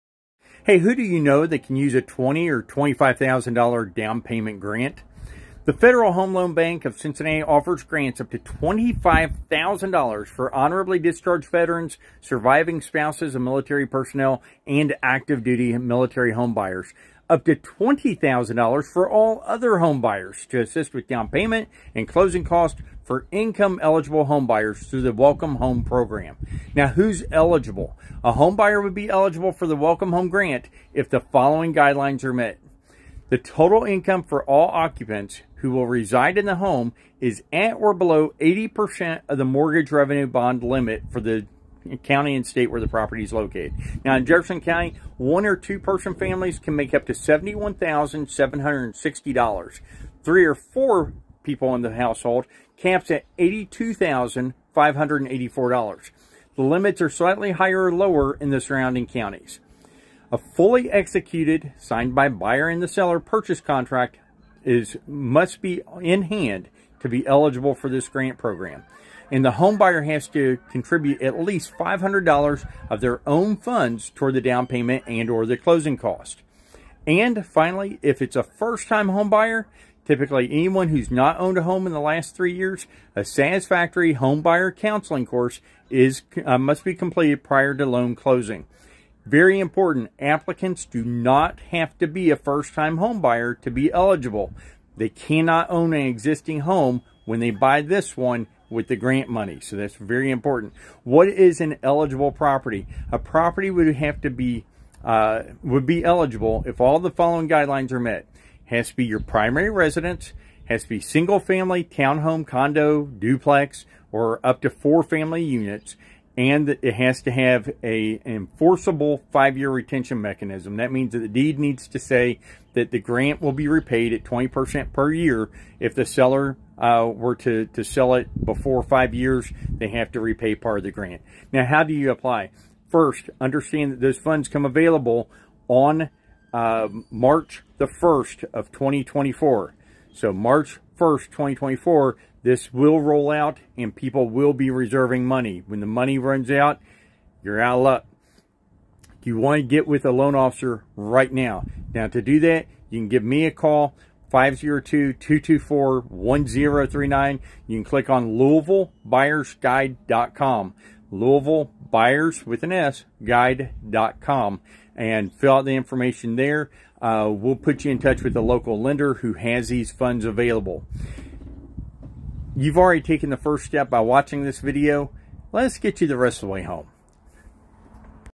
WELCOME HOME GRANT PROGRAM 2024 - Mark Atteberry, Louisville Market Realtors
Louisville Market Realtors is always searching for the best programs, grants, tax credits and down payment assistance for our clients. This is a summary of the 2024 Welcome Home Grant up to $25,000 offered by FHLB of Cincinnati and facilitated by qualified area banks like Wilson & Muir Bank (the 2nd oldest bank in Kentucky).

Beginning March 1, 2024, the Federal Home Loan Bank of Cincinnati offers grants of up to $25,000 for honorably discharged veterans, surviving spouses of military personnel, and active duty military homebuyers and up to $20,000 for all other homebuyers to assist with down payment and closing costs for income eligible homebuyers through the Welcome Home Program.

Who are Eligible Homebuyers?
A homebuyer would be eligible for the Welcome Home grant if all of the following guidelines are met:

The total income for all occupants who will reside in the home is at or below 80 percent of the Mortgage Revenue Bond limit for the county and state where the property is located – In Jefferson County 1 or 2 person families can make up to $71,760. 3-4 persons in the household cap at $82,584. The limits are slightly higher or lower in the surrounding counties.

A fully executed (signed by buyer and seller) purchase contract on an eligible property is in hand;

The homebuyer has at least $500 of their own funds to contribute towards down payment and/or
closing costs; And,

If a first-time homebuyer (typically anyone who has not owned a home in the last three years), a
satisfactory homebuyer counseling course is completed prior to the loan closing.

YOU DO NOT HAVE TO BE A FIRST-TIME BUYER TO OBTAIN THIS GRANT.

What is an Eligible Property?
A property would be eligible if all of the following guidelines are met:
The property will be the homebuyer’s primary residence;

The property is a single family, townhome, condominium, duplex, multi-unit (up to four family units);

And, The property is subject to a legally enforceable five-year retention mechanism – this means it is forgiven at a rate of 20% per year and if you sell before 5 years, they will be repaid part of the grant.

How Do You Apply?
We’ll put you in touch with a local lender who has these funds available.

Search all the active listings at louisvillemarket.com

You’ve taken the first step, now let us get you the rest of the way HOME!